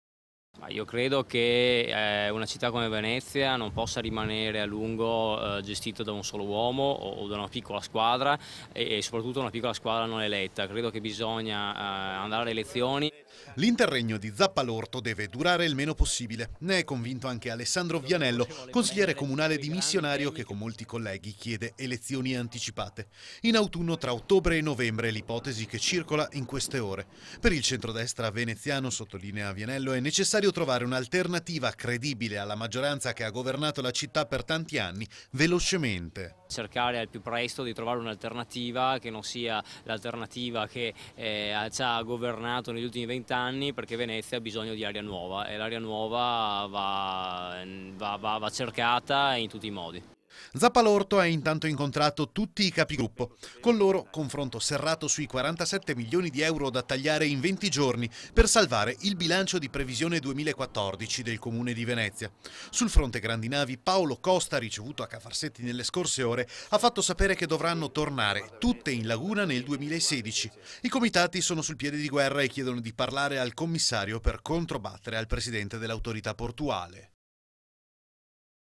12/07/2014 - TUTTI DA ZAPPALORTO...ASPETTANDO LE ELEZIONI
L'interregno di Zappalorto deve durare il meno possibile.
Ne è anche convinto Alessandro Vianello, consigliere comunale dimissionario, che con molti colleghi chiede elezioni anticipate.
In autunno, tra ottobre e novembre, l'ipotesi che circola in queste ore